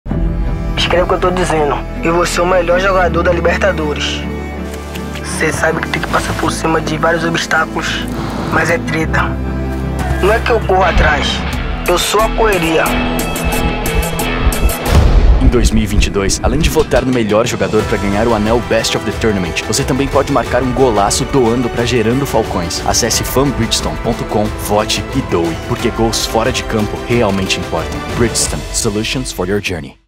Best of the Tournament 2022 | versão 30"
A votação OFICIAL é aqui e já está aberta, vote e faça parte dessa grande decisão!

Em 2022, além de escolher o Best of the Tournament, é a nossa chance de fazer a diferença na parceria com a Gerando Falcões. Confira como você pode participar e confira todo o histórico que elege o melhor jogador da CONMEBOL Libertadores a cada ano.